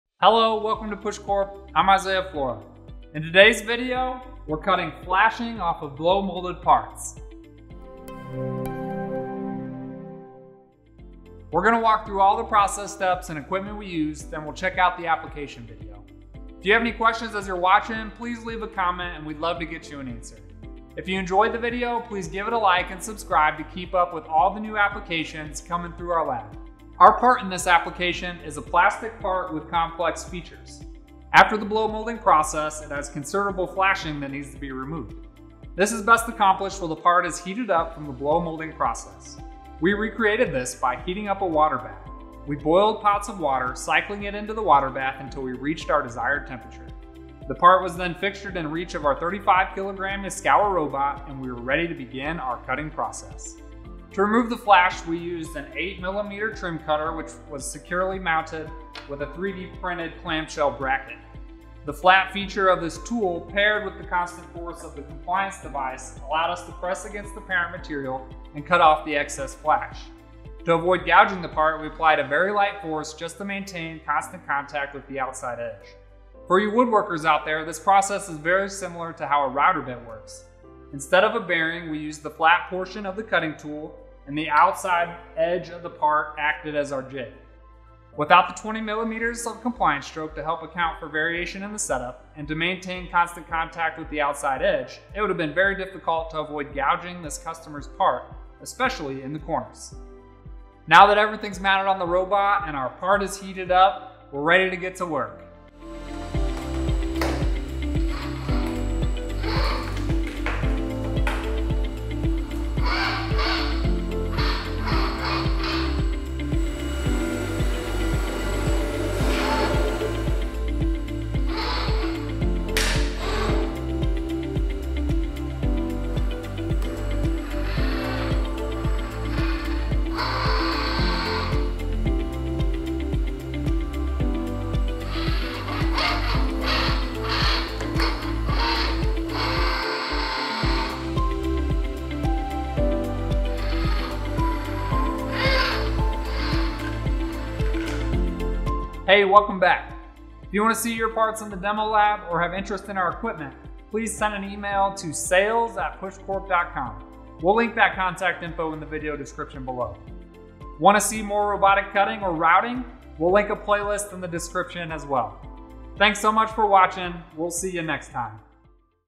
Plastic Trimming and Deflashing Automation With a Force Control EOAT and an Industrial Robot
In this video, we trim and deflash plastic blow molded parts using PushCorp Force Control end of arm tooling. This is paired with a Yaskawa 35 kg robot for highly repeatable results.

Unlike a force torque sensor, this device has 20 mm of compliant stroke to apply a constant force and account for fixturing and part variation. To learn more about this technology make sure to check out the following link.